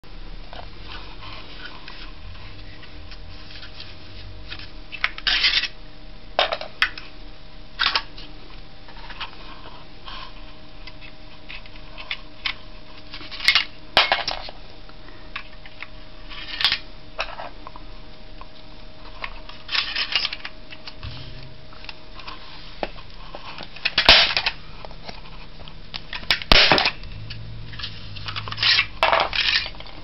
TEST такер Novus J-02AL - коване със скоби за кабел CT60
Големият хоби такер за стандартни работи от ABS пластмаса;
Подходящ за универсална употреба в градината и в къщи;
Идеален за закрепване на кабели с малко напрежение до 50V и диаметър до 6мм.;
Коване на обикновени скоби до 14мм. дължина;